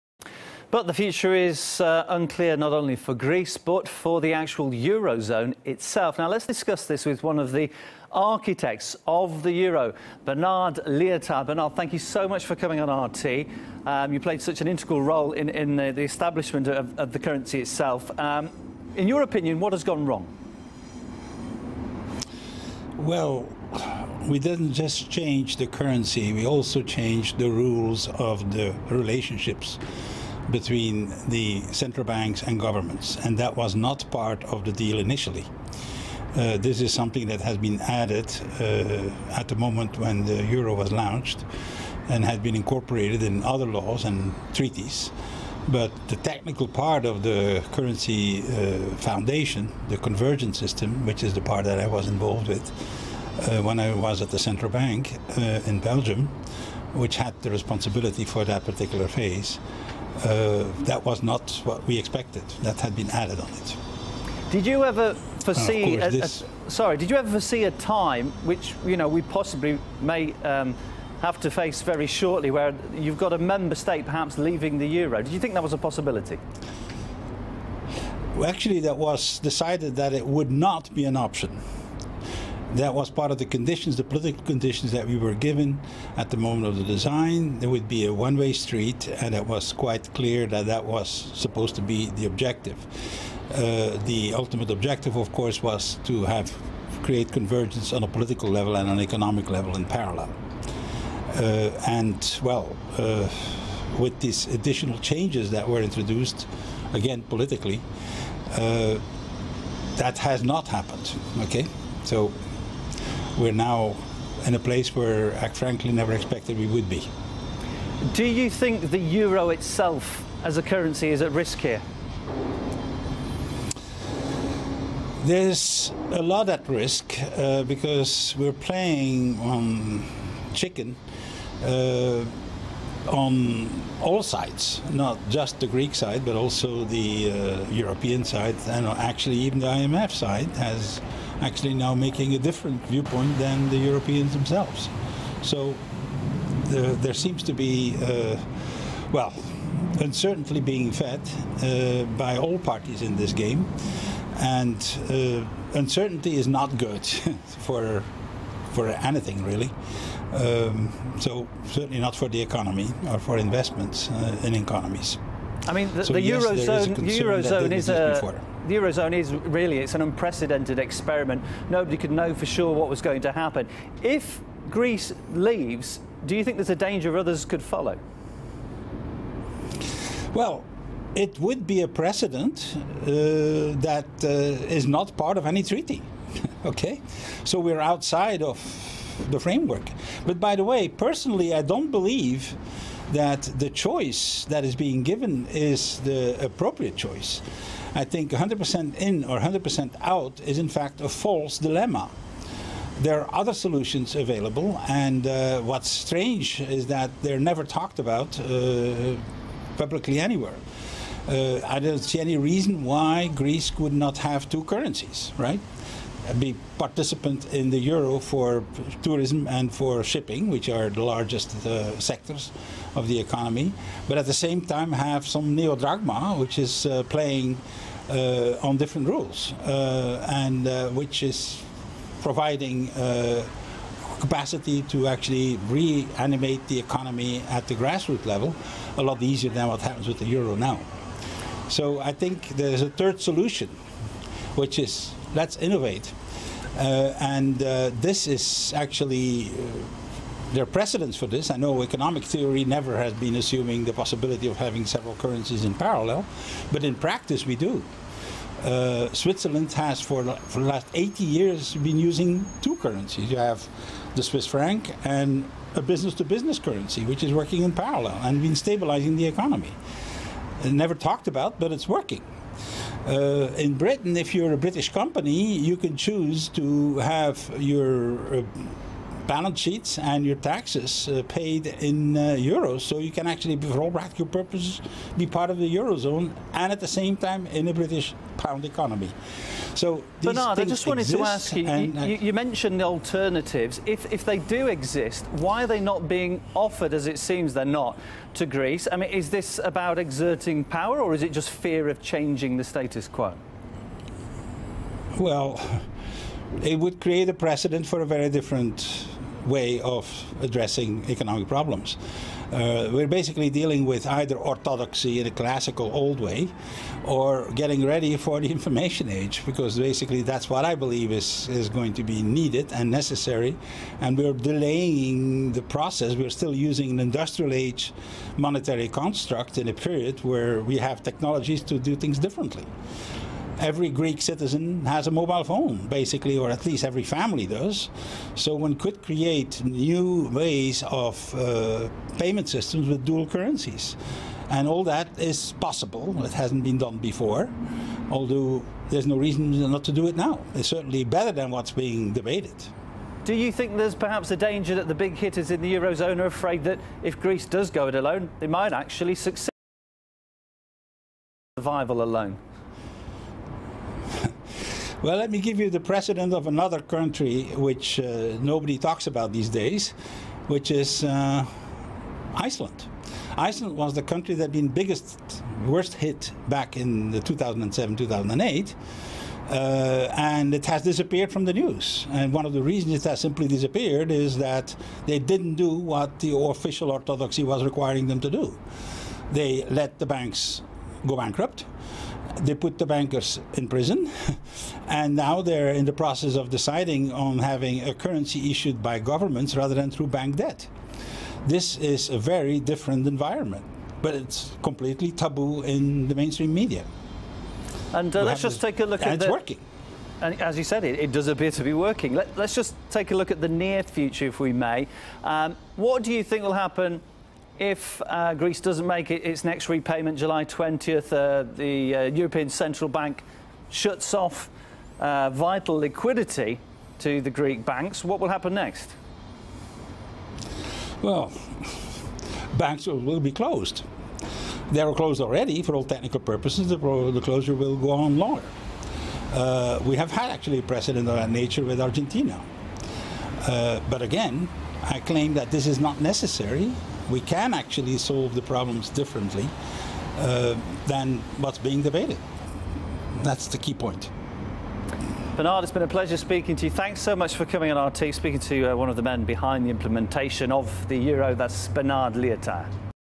Bernard Lietaer - Greece needs a complementary currency
Countries in financial crises could have two currencies in parallel that would stabilize the economy, Bernard Lietaer, co-designer of the predecessor of the Euro. "For eighty years Switzerland successfully used the Swiss franc and a so-called ‘business-to-business’ currency, he said. In Great Britain, domestic companies have the right to choose whether to have balance sheets and taxes paid in pounds or euro." - Lietaer

RT is kindly asked to leave this video here. References have been made crystal clear below, any further request for correction is kindly awaited:
- Original video from RT:
Title: 100 percent in/out choice given to Greece is ‘false dilemma’ – euro architect to RT
Original RT Published time: July 03, 2015 14:30
Original RT Edited time: July 03, 2015 16:41
- Partial transcript (the end is missing):

Another video created by RT with Bernard Lietaer on the Big Picture show: